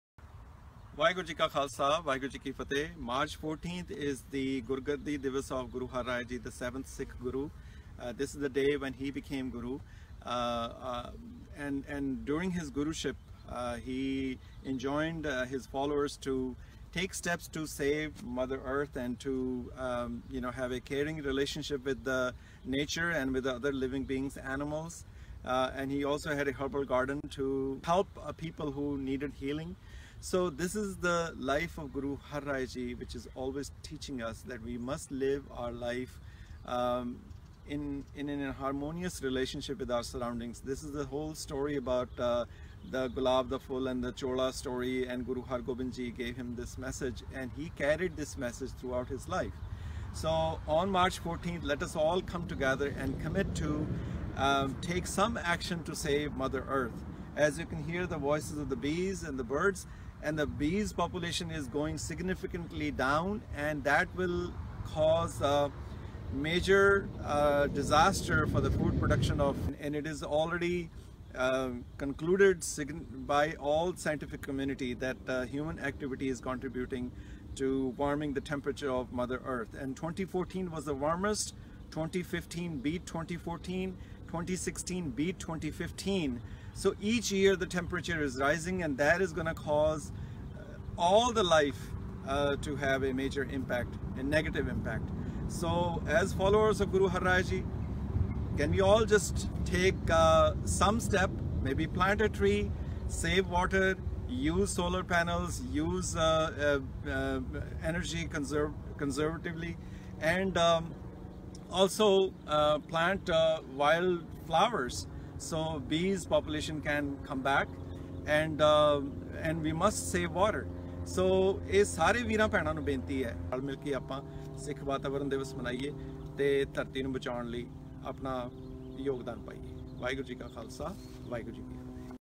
Rajwant Singh talking about the Sikh Environment Day
Rajwant Singh talking about the Sikh Environment Day and why it is important for Sikhs to take action on this day! Please share! ਵੱਧ ਤੋਂ ਵੱਧ ਸ਼ੇਅਰ ਕਰੋ।
Sikh Environment Day March 14 is celebrated all over the world.
This day, which marks the Gurta Gaddi diwas of Guru Har Rai ji, has been facilitated by EcoSikh since 2011. Plan an activity in your gurdwara, school or college and dedicate it to the 7th Guru who taught us to care for nature
Take 7 steps to celebrate the 7th Guru!
1) Green Diwan - organize kirtan & Katha on nature (shabads from Gurbani)
2) Plant trees around Gurdwara or nearby areas
3) Share Sakhis and stories about Guru Har Rai in Diwan
4) Organize a nature walk or any outdoor activity in nature
5) Buy & cook organically grown food at home and in Gurdwara
6) Take a step to save water and to increase recycling
7) Organize a talk by an environmental expert for awareness in Gurdwara, school or college